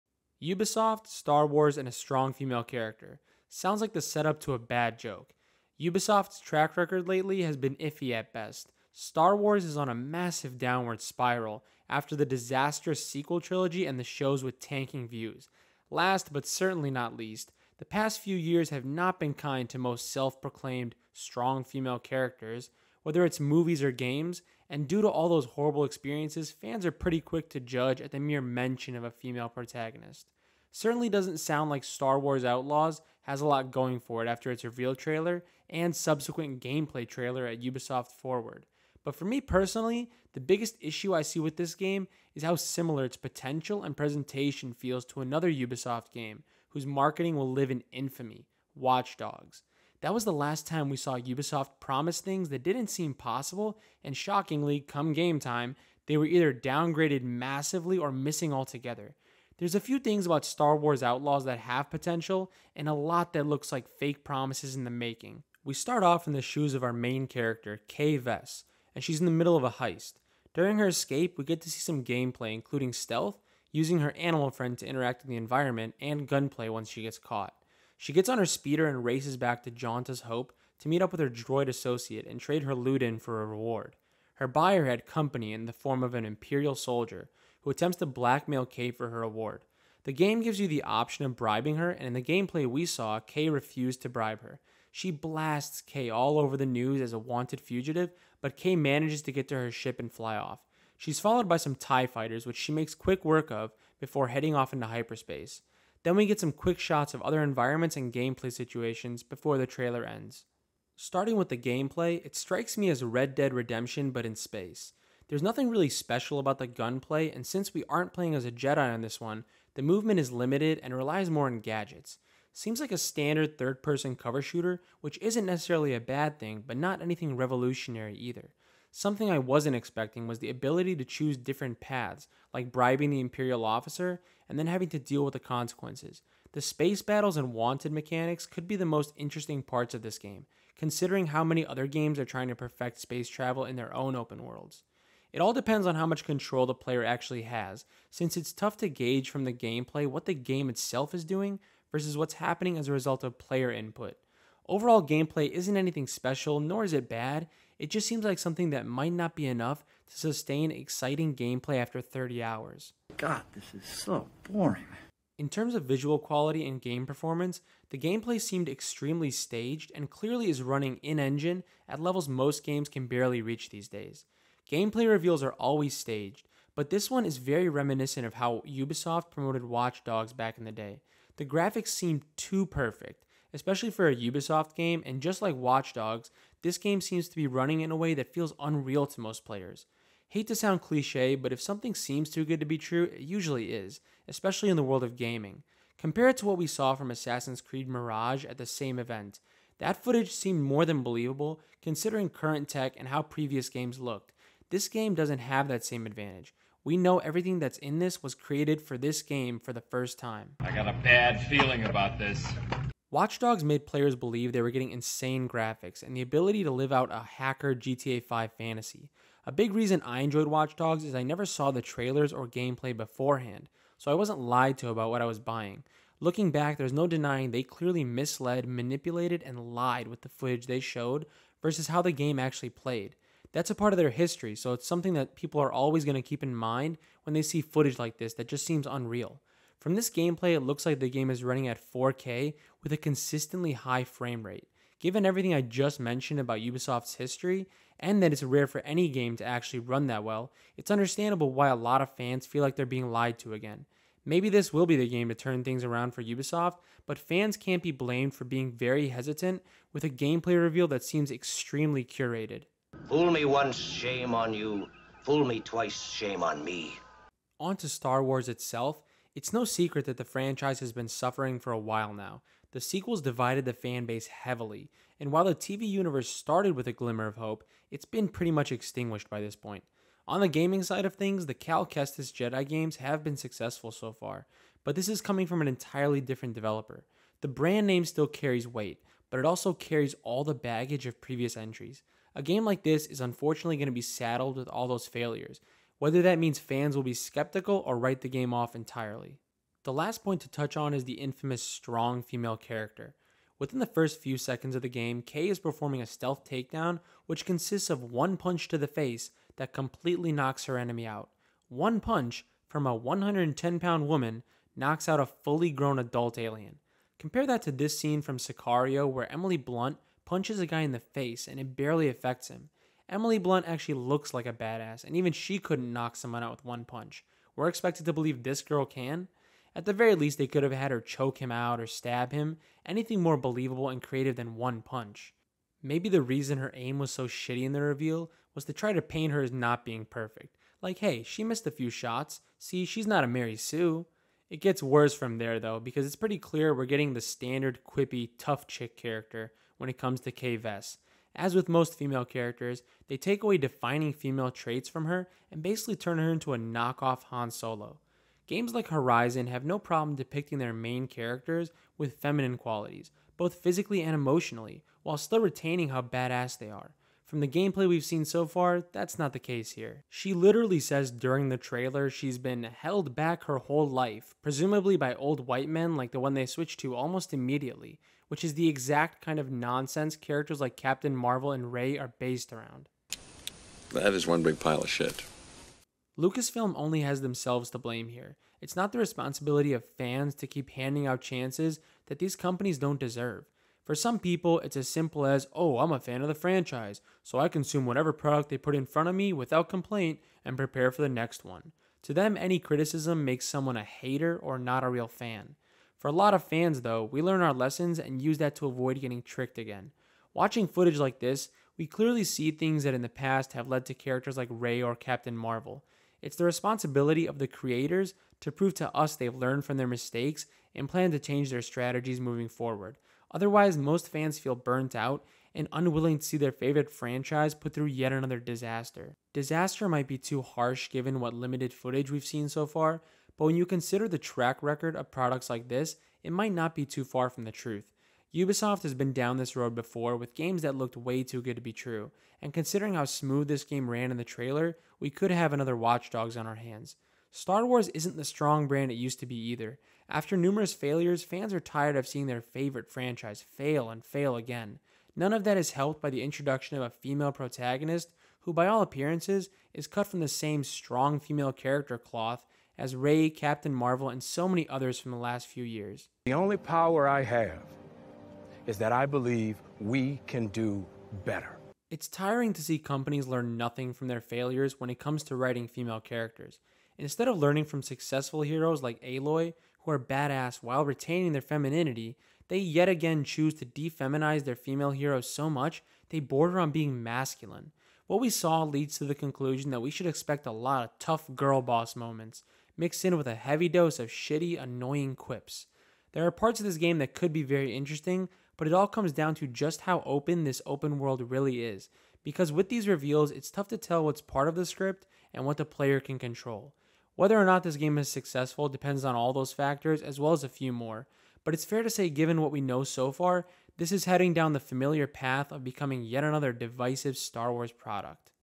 Star Wars Outlaws - I Have A Bad Feeling About This
Star Wars Outlaws is the latest entry to the franchise to be announced, and we got the Star Wars Outlaws Trailer and gameplay within a few days. Suffice to say this new Star Wars open world game has a lot of people skeptical about what they're promising especially given Ubisoft's past.

The Star Wars Outlaws gameplay trailer showed off some basic gameplay situations, and while it looks good Ubisoft has a history of manipulating their test footage to lie about the quality of a game. This new Star Wars open world game just looks too good to be true, and plot wise there are more than a few indications that we'll have to deal with the same nonsense that already ruined Star Wars and a ton of other franchises as well.